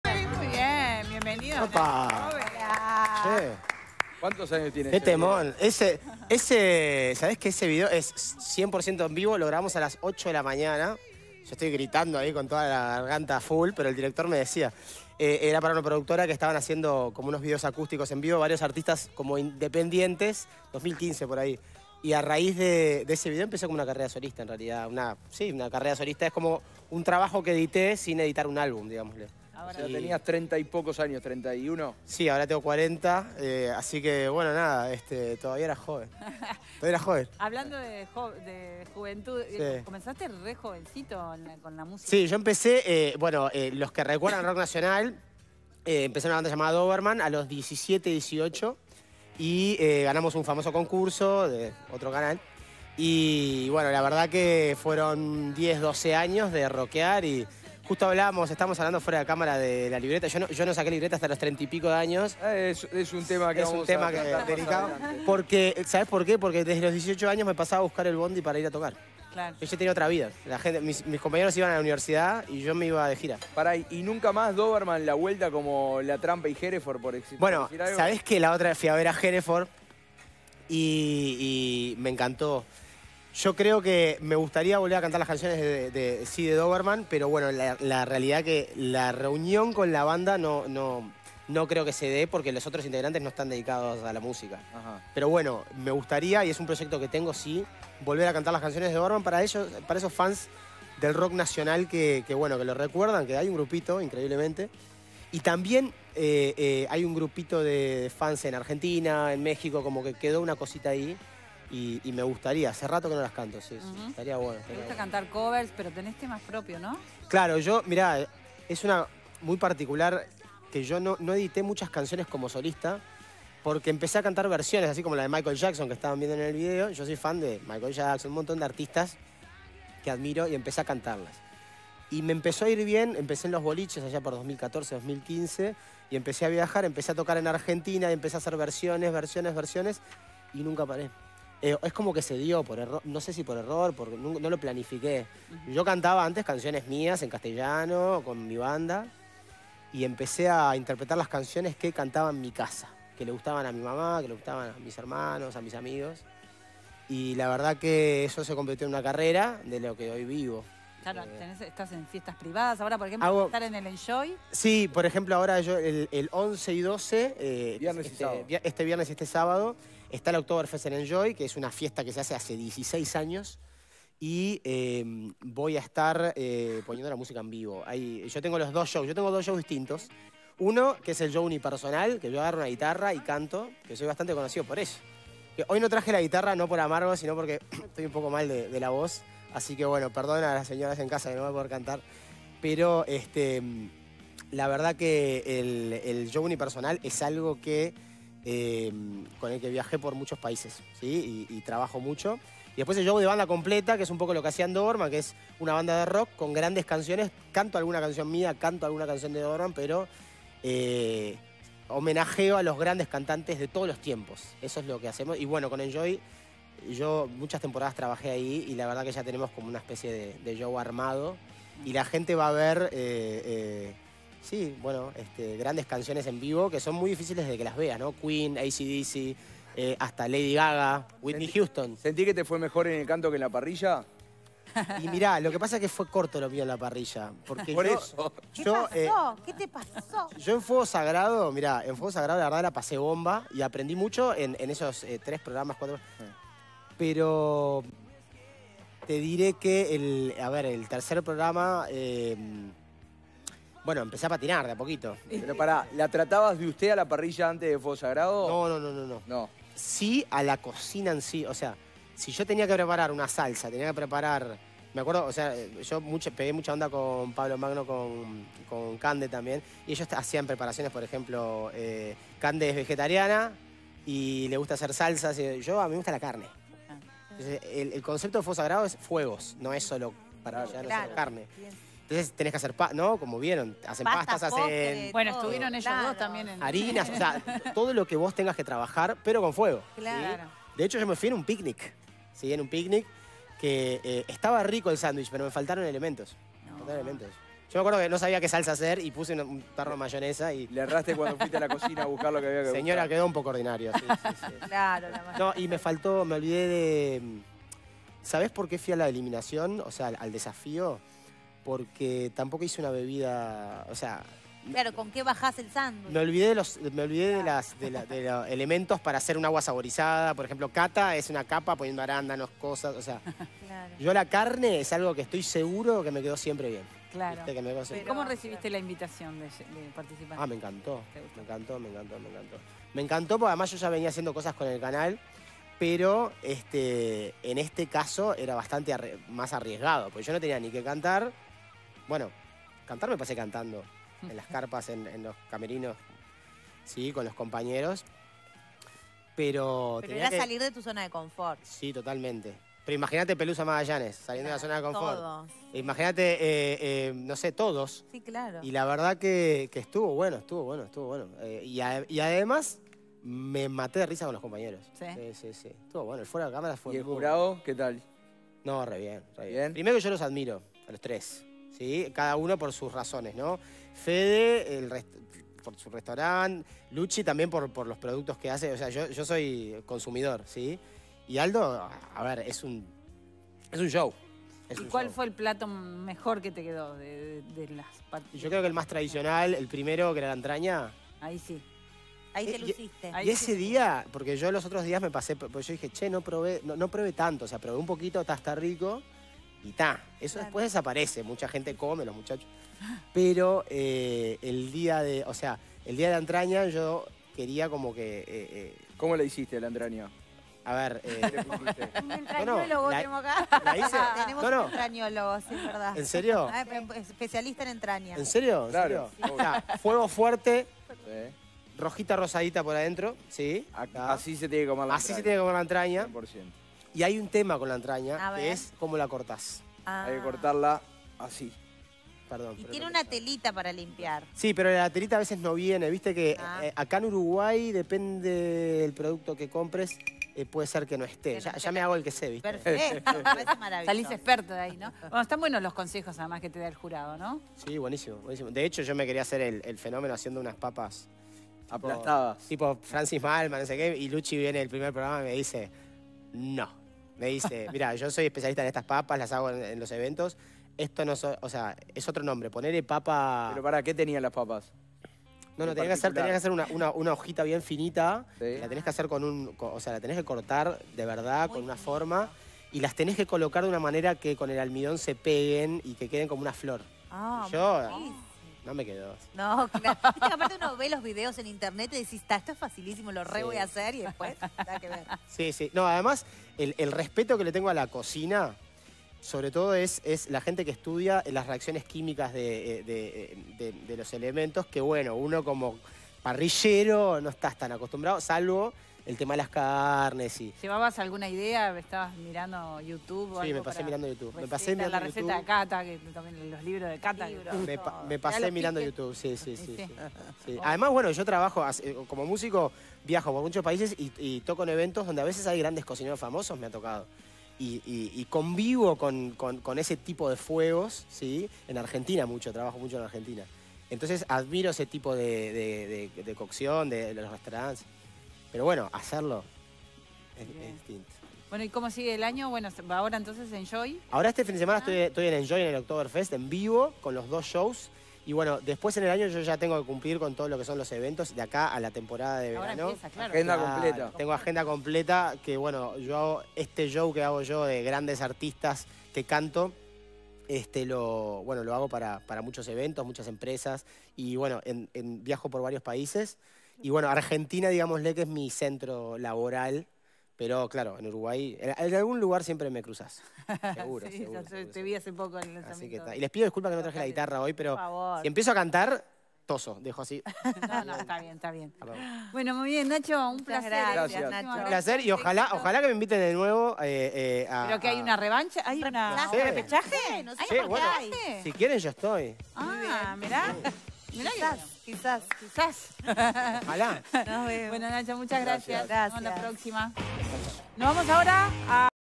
Los comienzos en la música de Nacho Obes
Se presentará el viernes 11 y sábado 12 en la oktoberfest de Enjoy